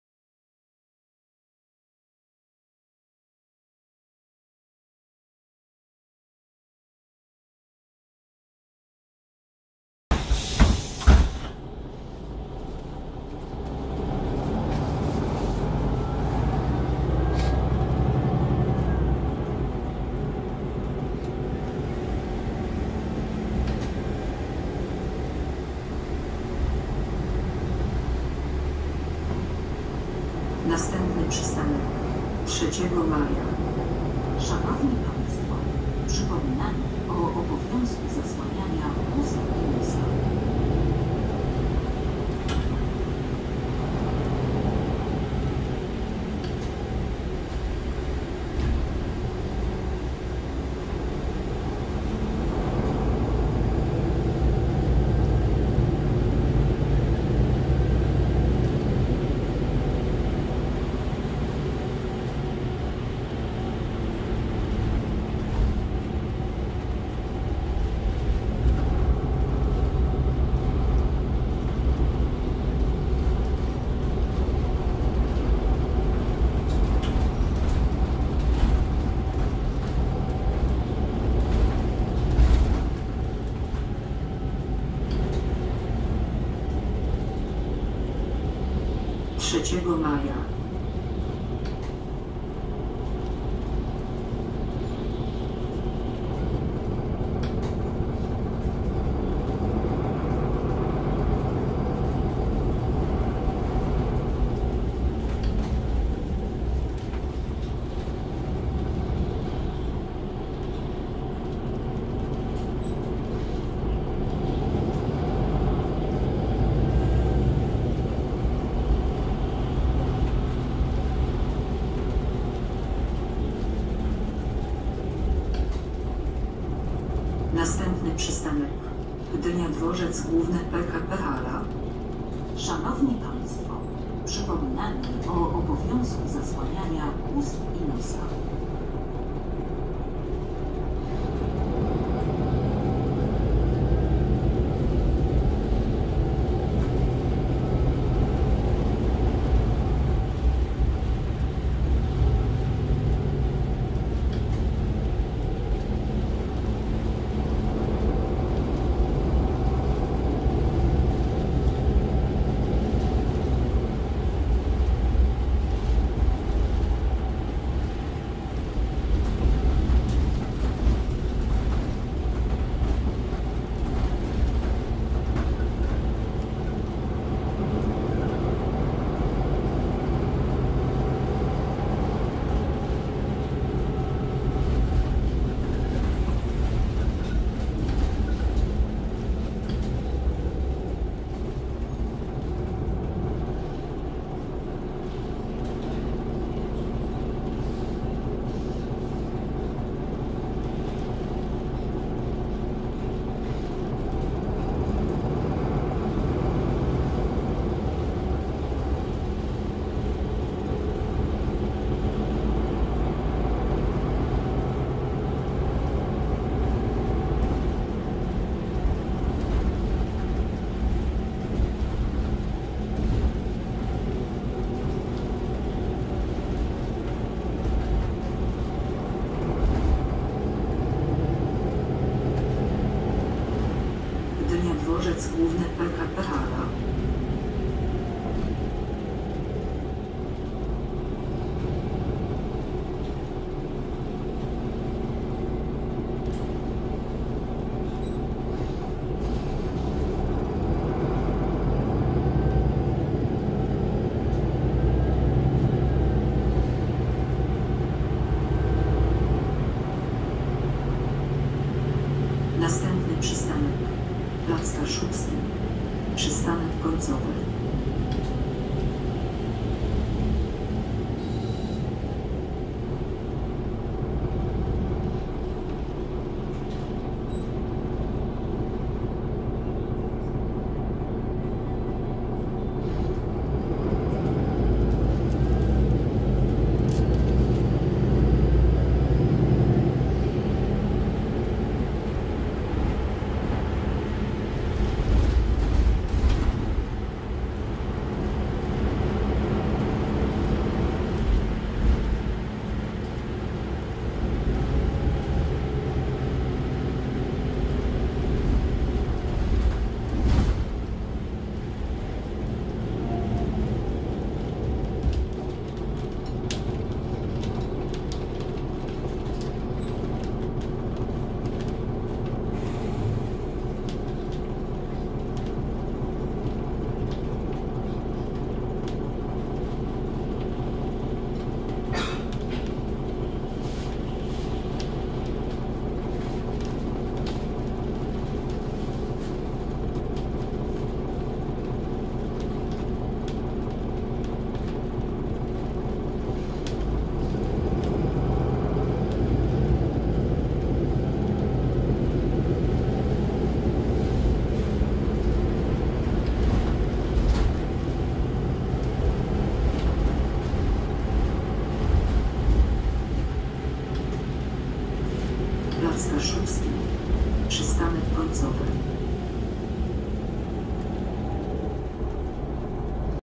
PKM Gdynia - MAN NL283 Lion's City #2029 | linia 190
Tytułowy autobus pochodzi z ostatniej dostawy nowych MANów dla Gdyni. Co ciekawe autobusy zostały rozdzielone na obie zajezdnie, a nie tylko na Kacze Buki, która słynęła z autobusów tej marki. Są to ostatnie lwy z serii Lion's City III generacji.
Info;
Rocznik/Year of production: 2019
Rok nagrania/Year of record: 2021
Trasa/Route:
Armii Krajowej
3-go Maja
Gdynia Dworzec Główny PKP - Hala
Plac Kaszubski koniec trasy/final stop